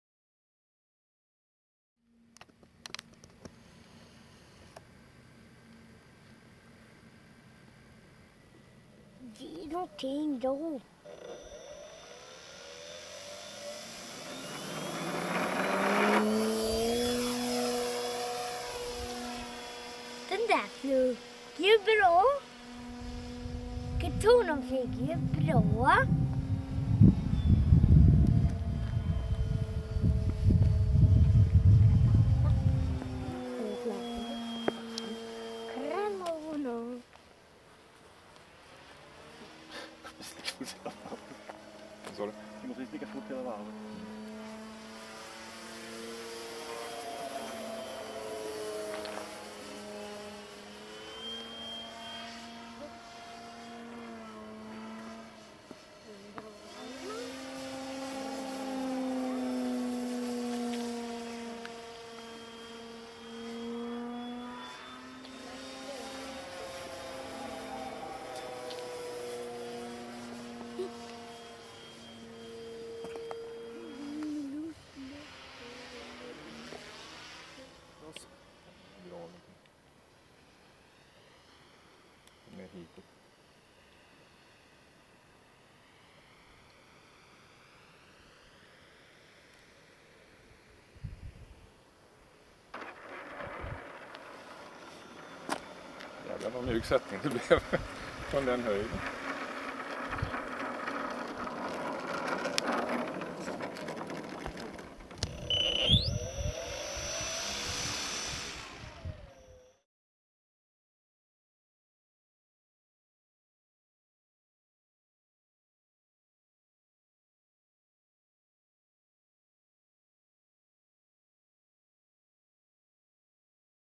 100829-Provflygning-Katana.wmv
Jocke provflyger sin Sebart Katana.